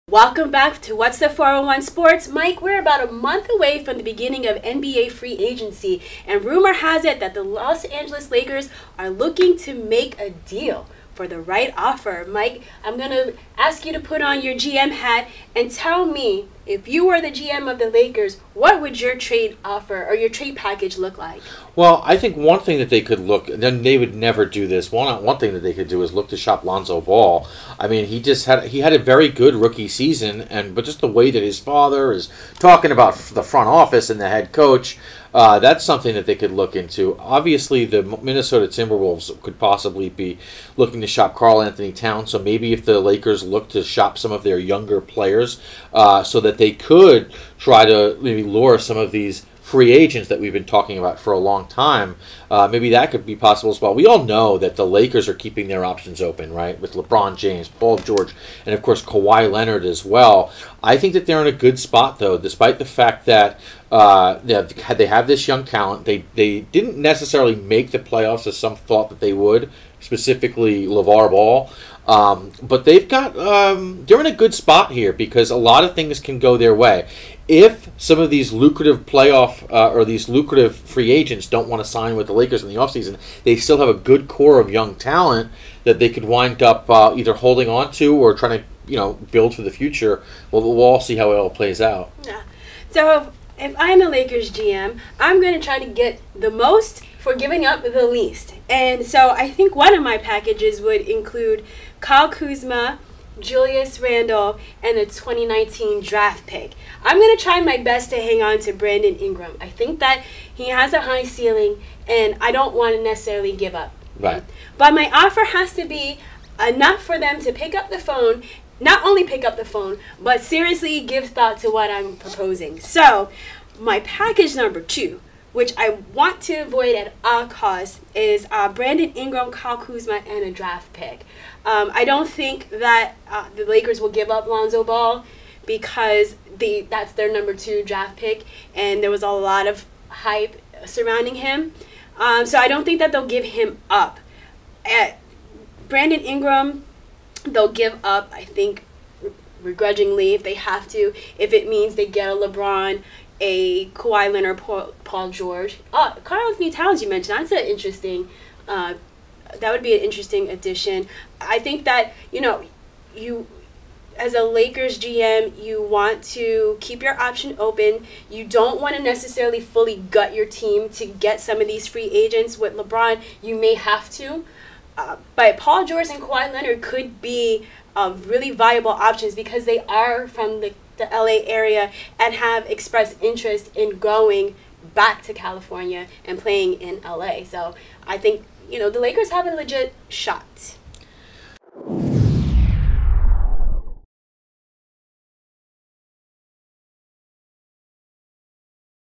Will The Los Angeles Lakers Make Major Moves in the Off-Season? | What's The 411Sports | NBA
As the NBA season ends and preparations are underway for NBA Season 2018-19, eyes are on the Los Angeles Lakers as rumor has it that the team may be willing to trade top players for top-tier players.